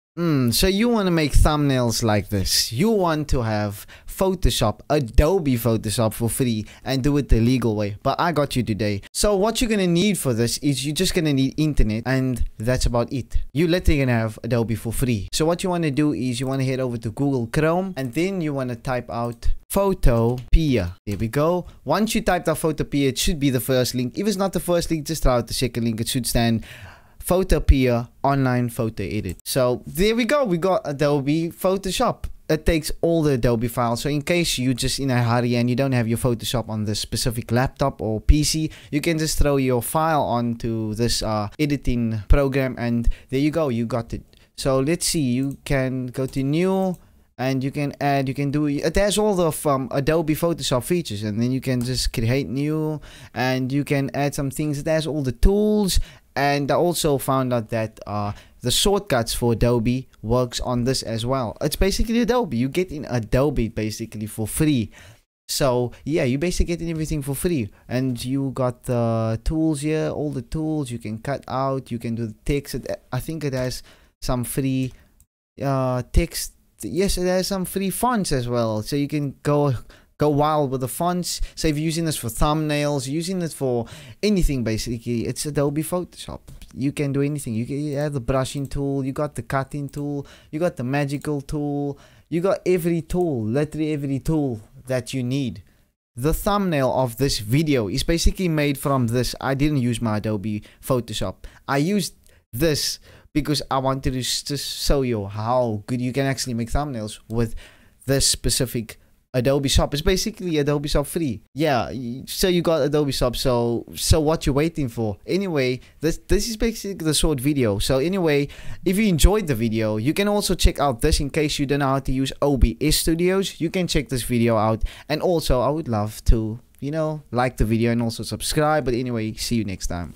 How To Get Photoshop For FREE!! | (Photoshop Alternative 2022)
What's up guys! Today I'm going to show you how to get photoshop for free (2022). Follow these simple steps to get started! Godspeed!

🎥: MY EQUIPMENT:
-Gaming PC
*Ryzen 3 3200g
*GTX 970
*16gb ram TForce
*Dixon Mouse
-Microphone
Samson CO1UPRO